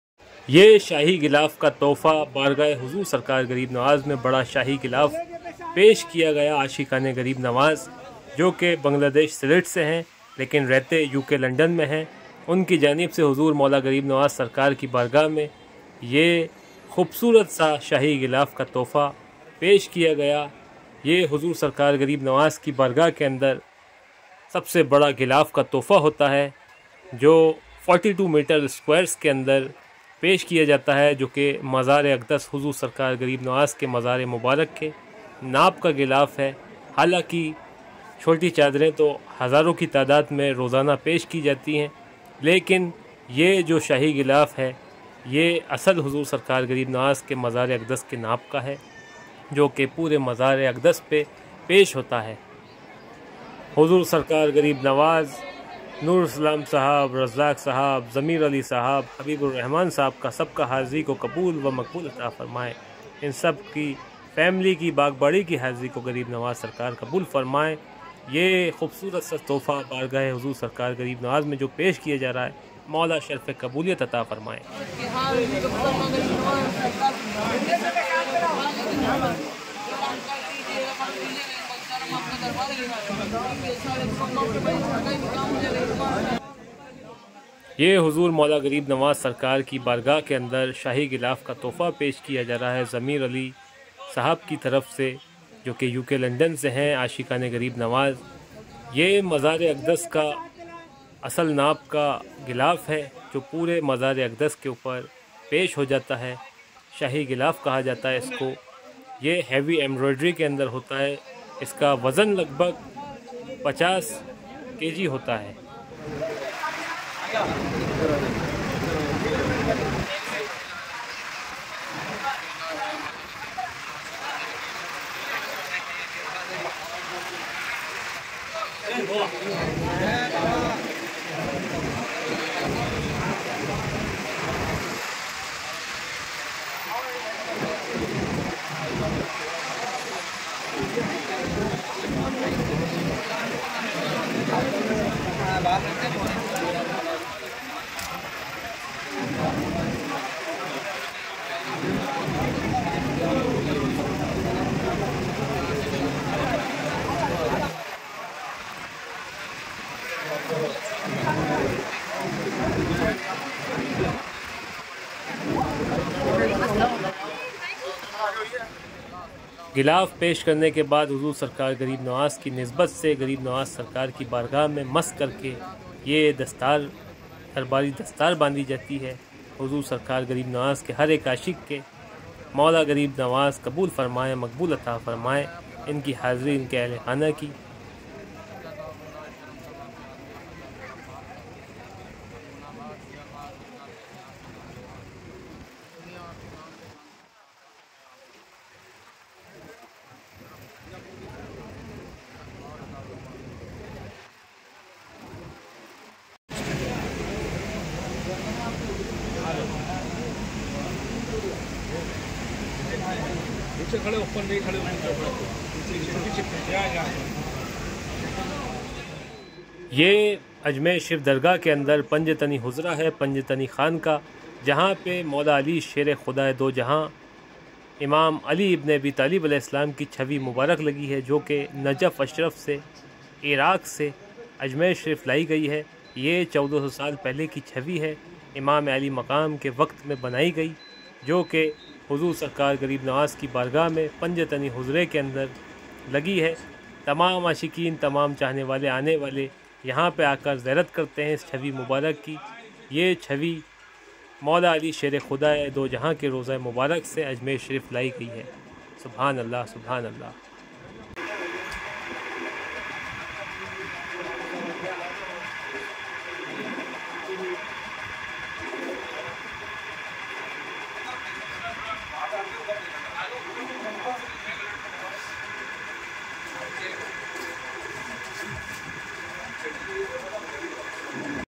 Shahi Ghilaaf Chadar Poshi Ajmer Sharif Darbar e Huzoor Khwaja Garib Nawaz R.A Ajmer Sharif Dargah
SHAHI GHILAAF PESH KIYA GAYA CHADAR POSHI DARBAR E HUZOOR MAULA KHWAJA GARIB NAWAZ A.S DARGAH SHARIF AJMER